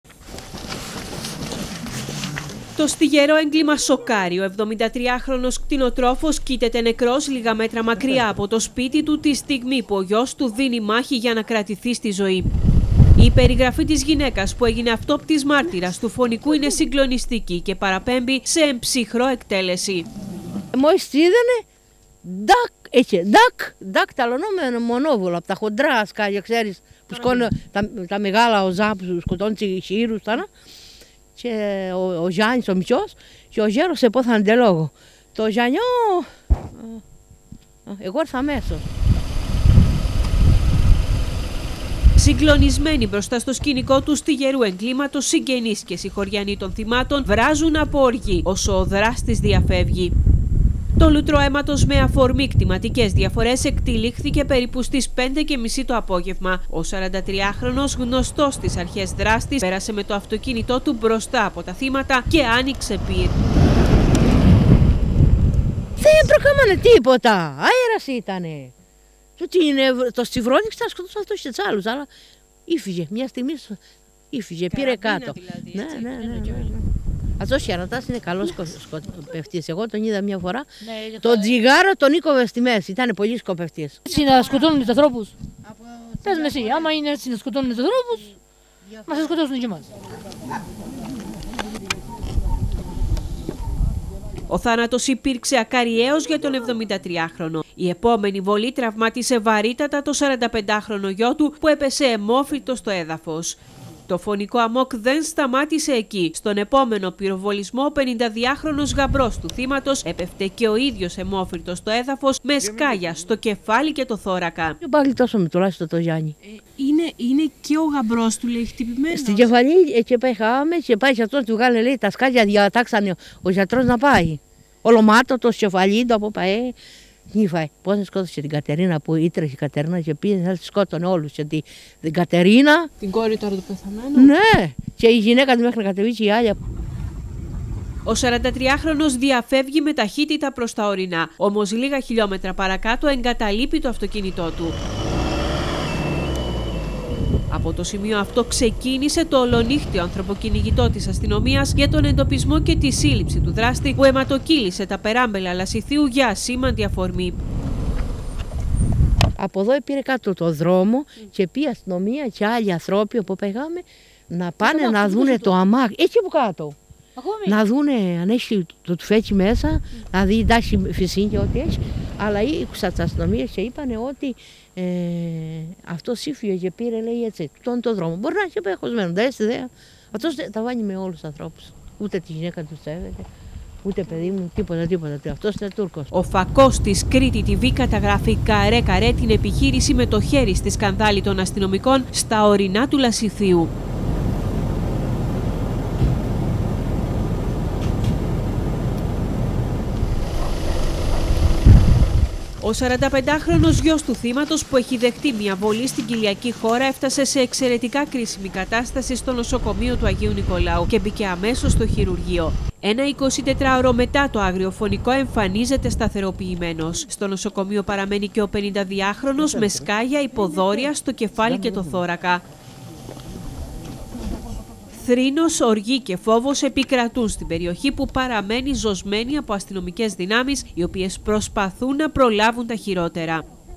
Αναζητείται ο δράστης του μακελιού στην Νεάπολη
διαβάστε επίσης:
Ασήμι: Οργή για τις σκηνές άγριας… Δύσης – «Σε πεδίο μάχης νταήδων η περιοχή» t.ly/W3zLv